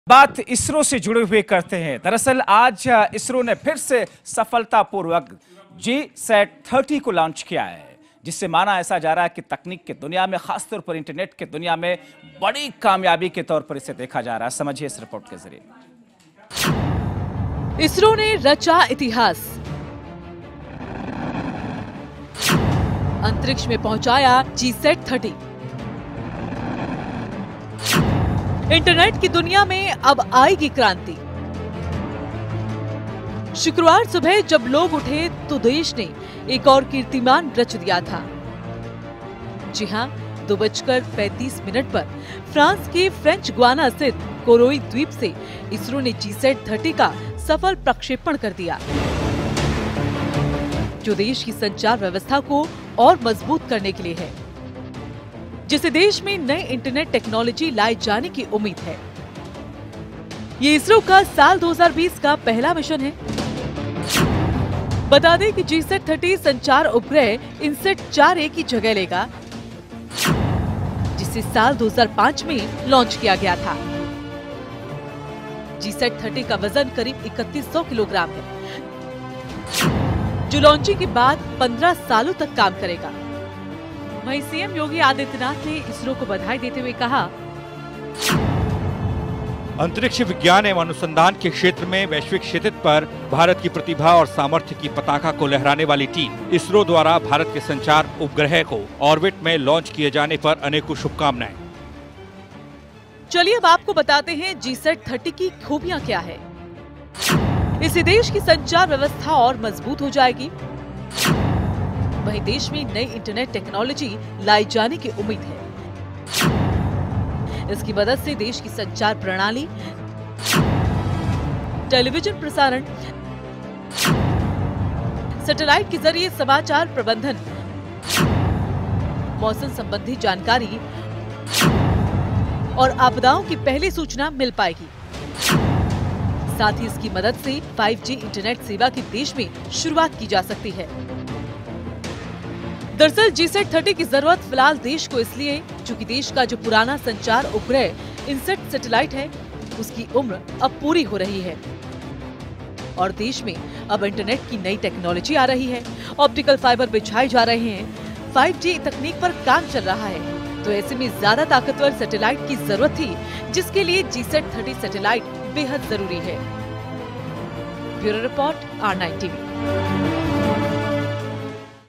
ISRO ने रचा इतिहास, अंतरिक्ष में पहुंचाया GSAT-30
इसरो ने अपना सबसे ताकतवर संचार उपग्रह जीसैट-30 सफलतापूर्वक लॉन्च कर दिया है...जिससे आने वाले वक्त में इंटरनेट की दुनिया में क्रांति आने की उम्मीद जताई जा रही है...